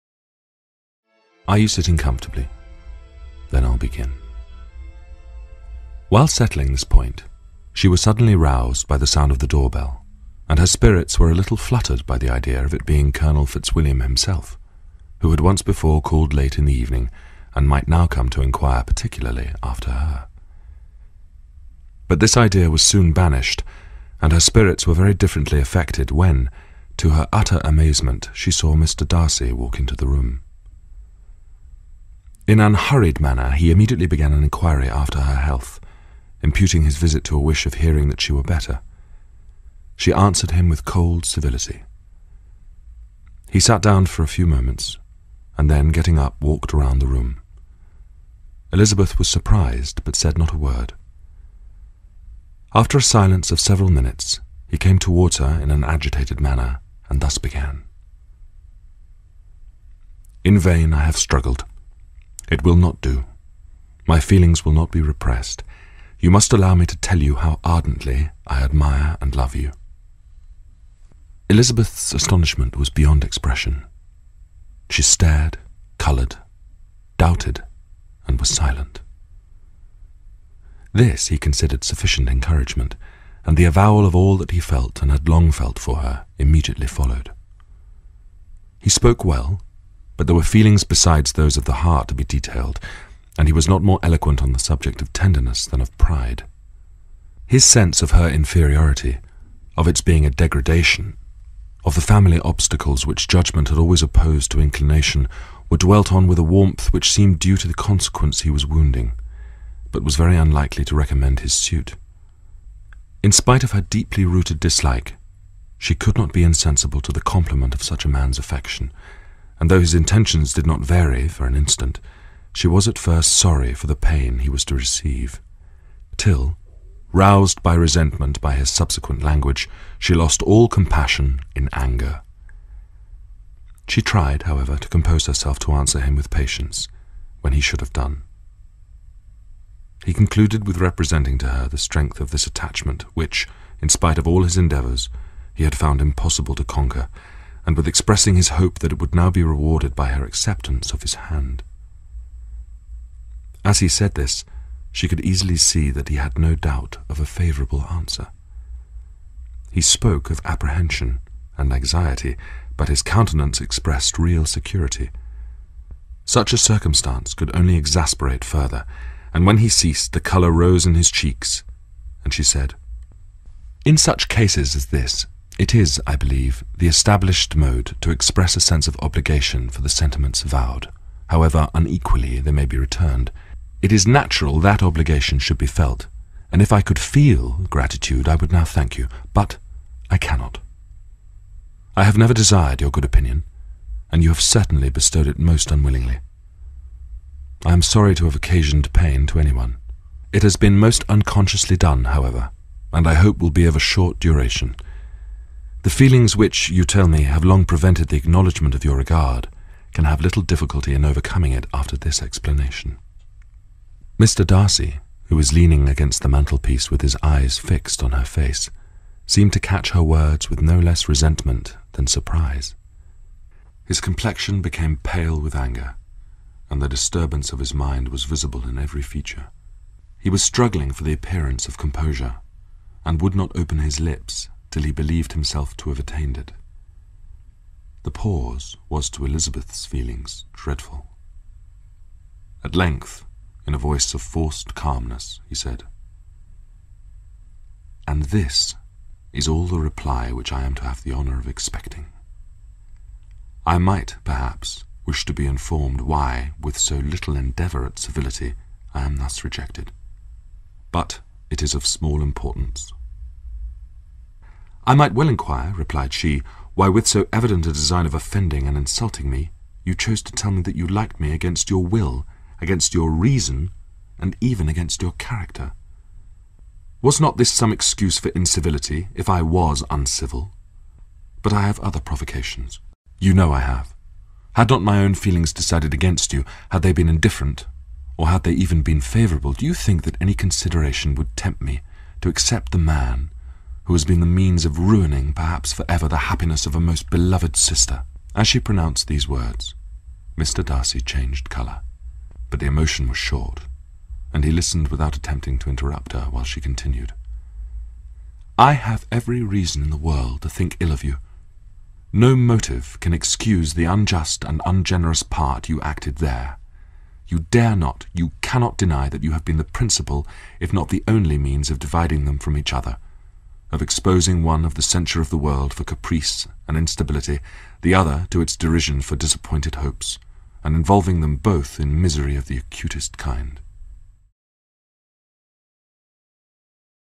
Matthew Macfadyen reads Pride and prejudice.Part 1.
Dedicated to people like me who love the great actor Matthew Macfadyen and especially his amazing voice. Matthew reads the first proposal passage of Jane Austen's book Pride and Prejudice.